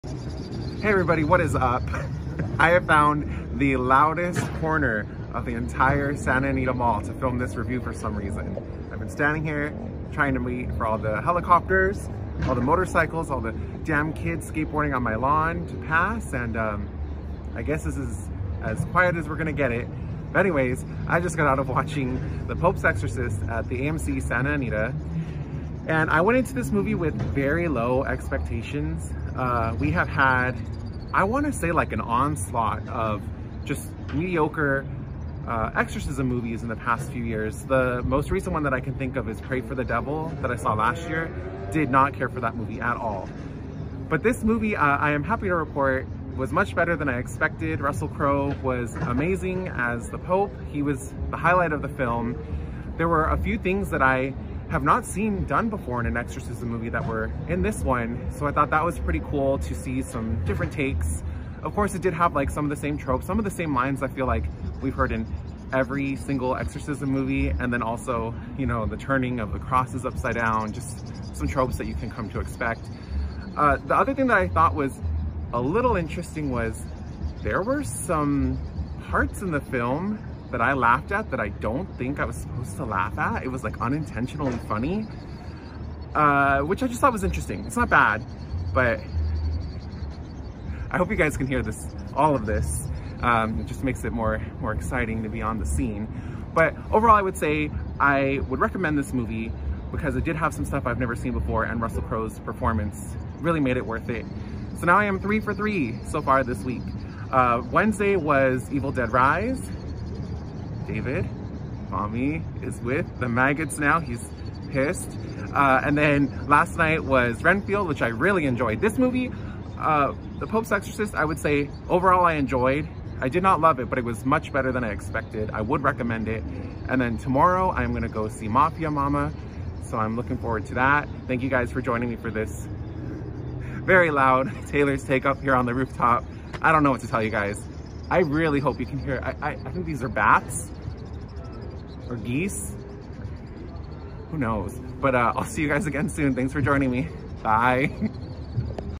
The Pope’s Exorcist Review
Hey everyone! Just got out of watching The Pope’s Exorcist at the AMC Santa Anita. Here is my spoiler-free “Taylor’s Take” on the film. Sorry for all the noises, I tried lol!